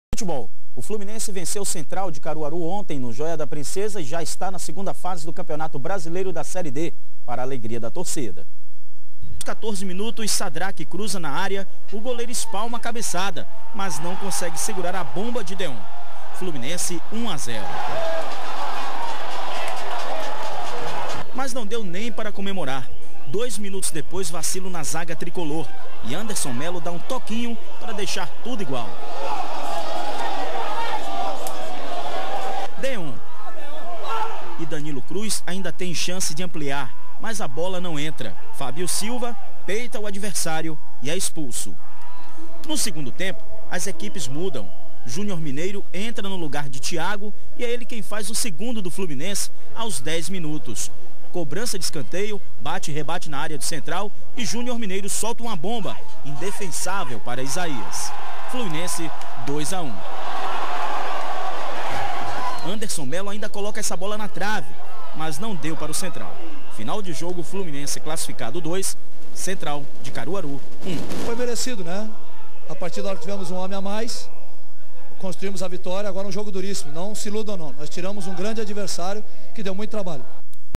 Flu de Feira vence o Central e se classifica para a 2ª fase da Série D
Imagens: TV Subaé